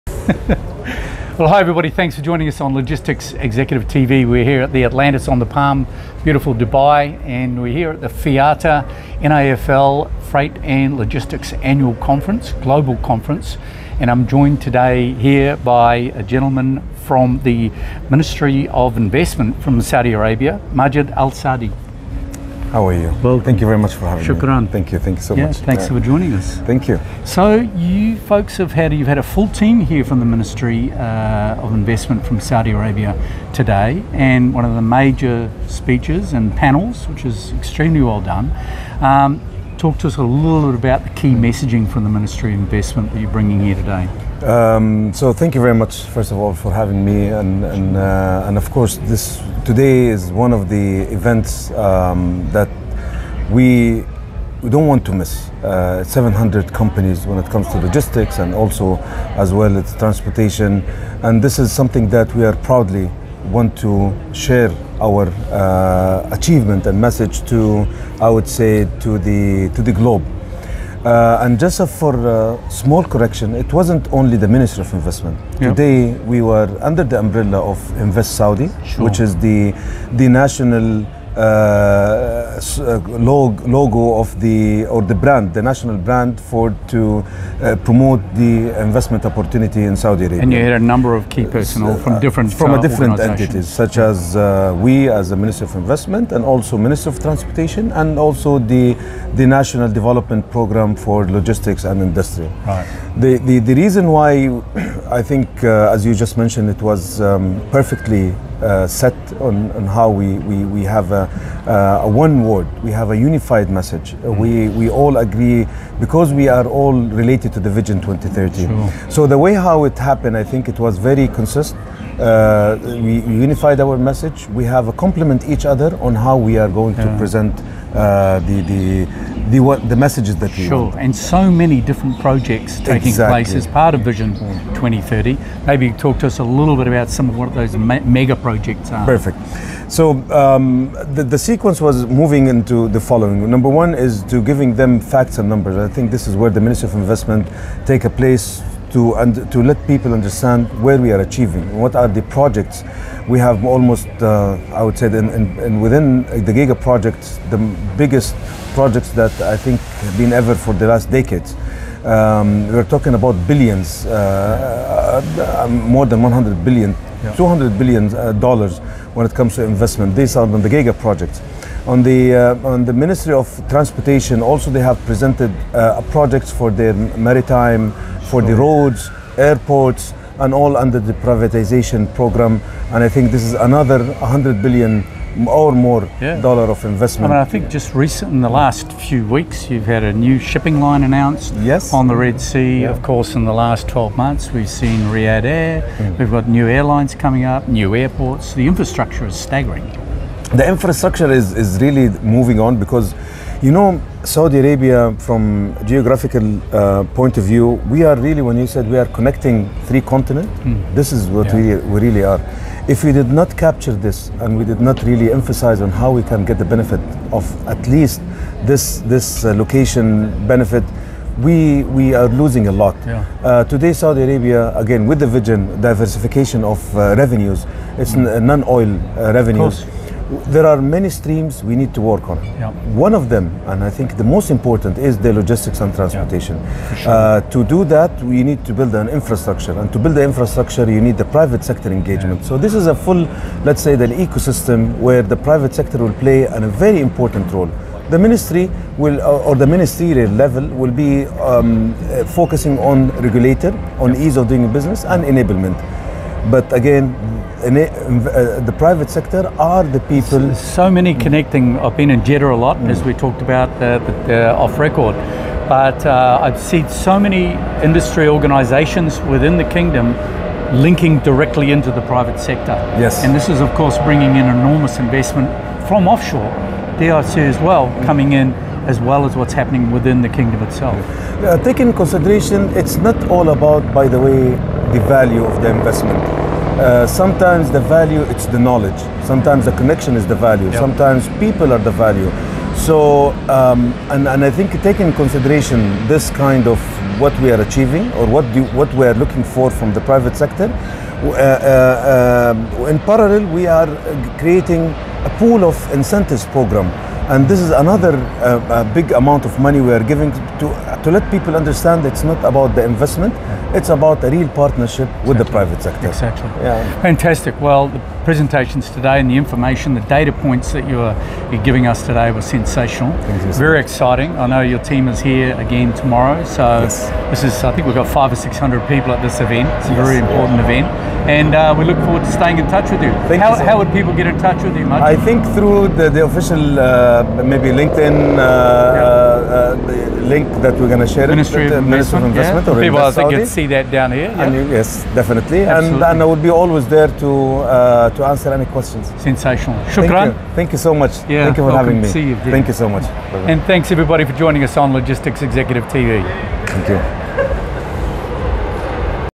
Event Series: FIATA Rame 2024 - Majed Al Saadi
Join us for a 6-minute chat with Majed Al Saadi General Manager of Investment Attraction Ministry of Investment @ the FIATA International Federation of Freight Forwarders Associations National Association of Freight and Logistics - NAFL RAME conference on March 5 & 6 2024.